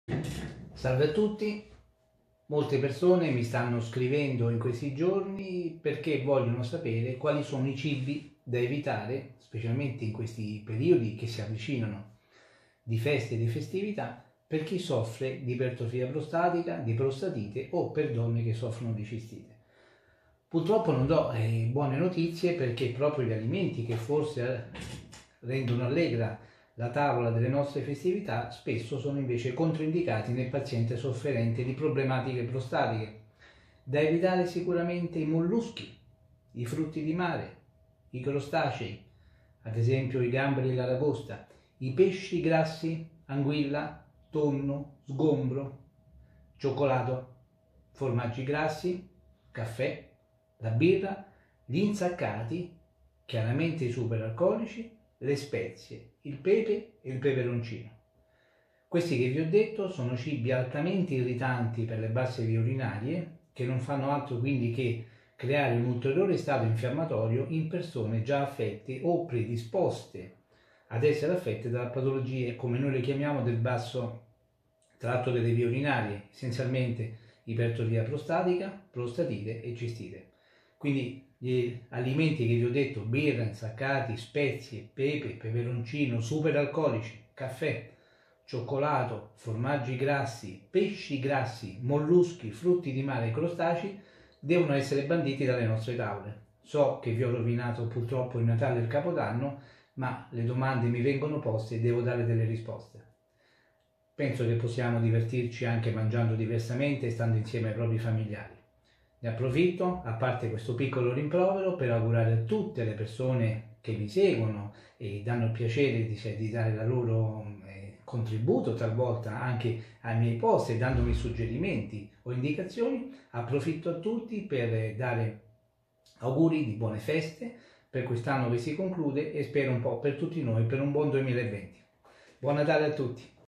Cibi da evitare per chi soffre di prostatiti e ipertrofia prostatica. Dr Andrea Militello Urologo
Spesso , specialmente quando si avvicinano le festività, i pazienti mi chiedono cosa possono e non possono mangiare nei vari cenoni che ci aspettano. ecco poche ma ferree regole ! Mi raccomando beviamo almeno due litri di acqua al giorno